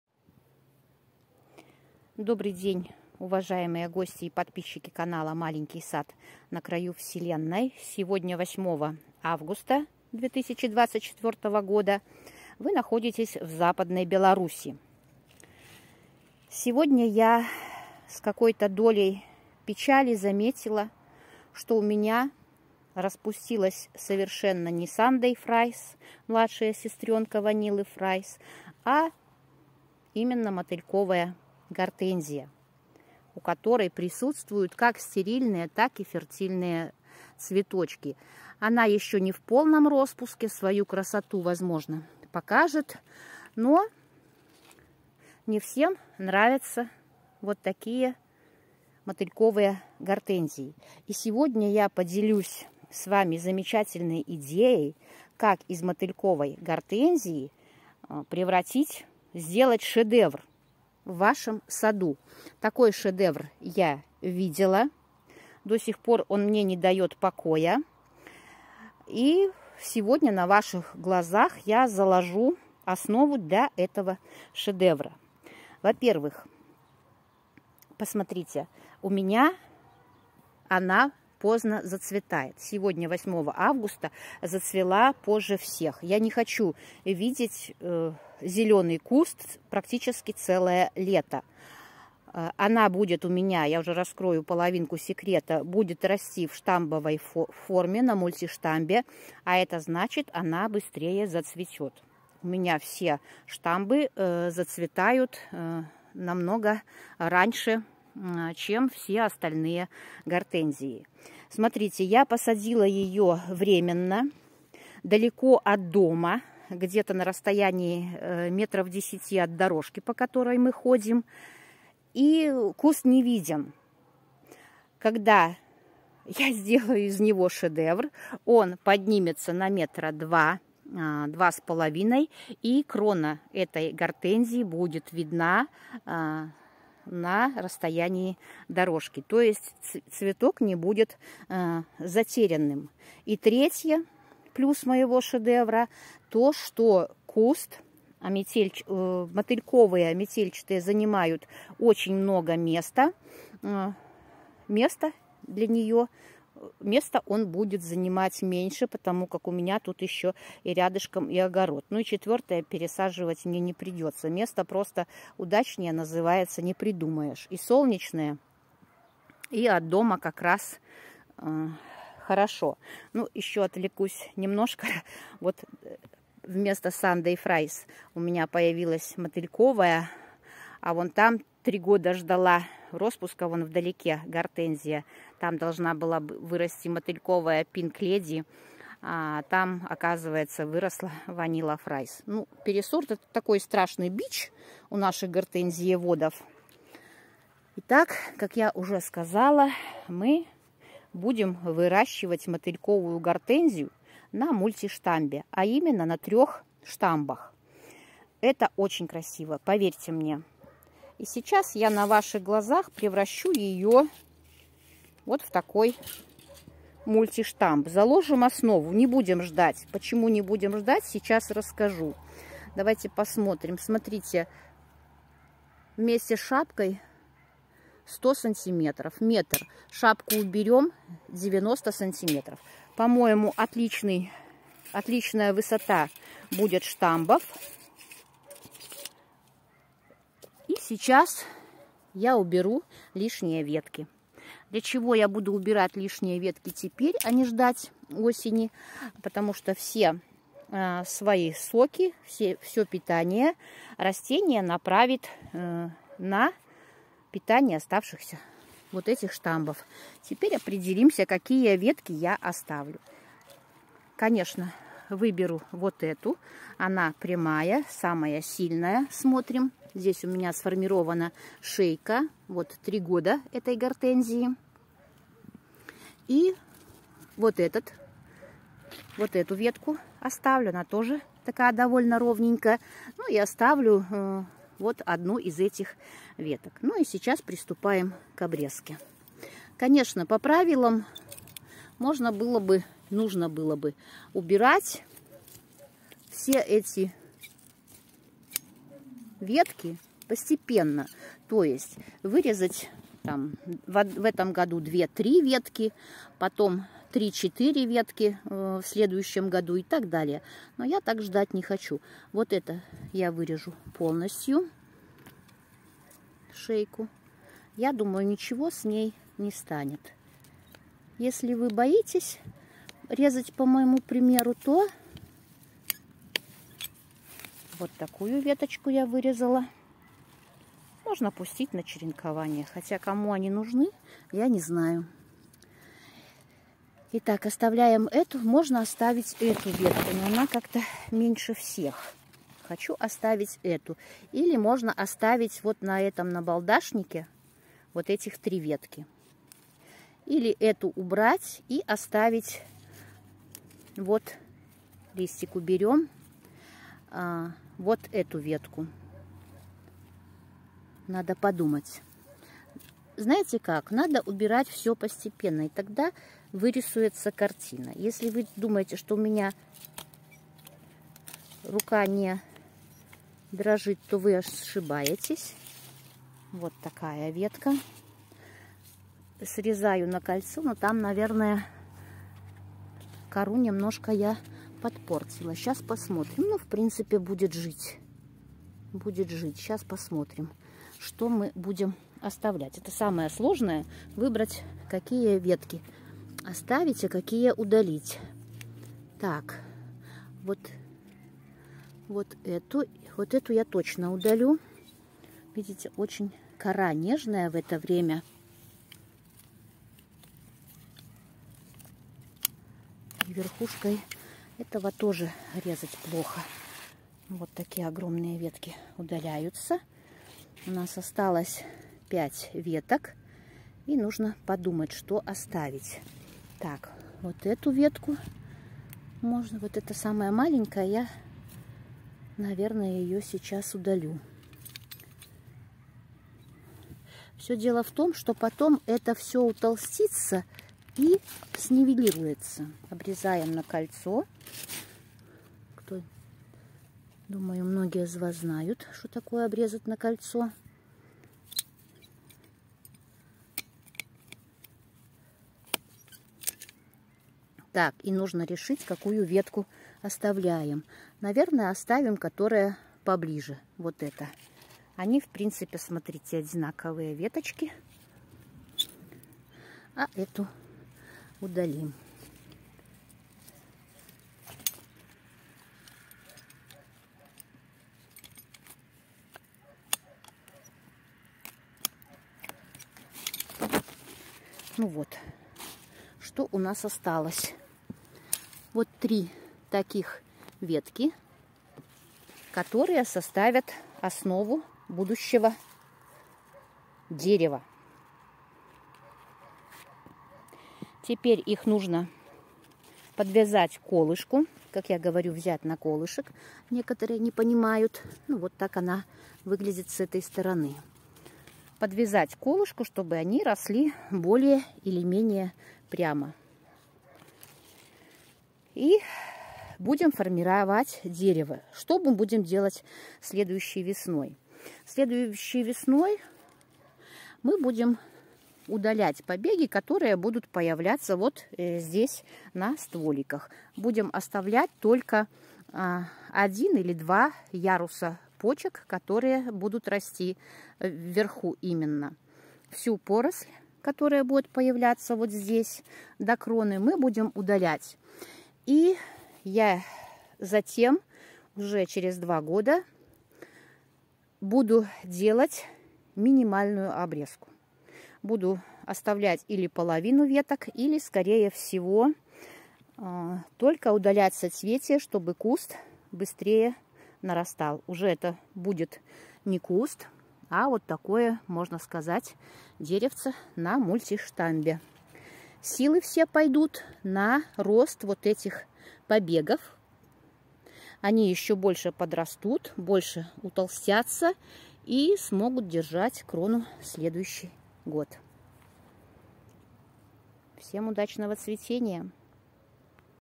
Идея для тех, кому не нравятся мотыльковые гортензии. Превращаем их в шикарное украшение сада
Мотыльковые сорта гортензий как никакие другие подходят для выращивания на штамбе. Идея - формируем мотыльковую гортензию на мультиштамбе в виде огромного дерева.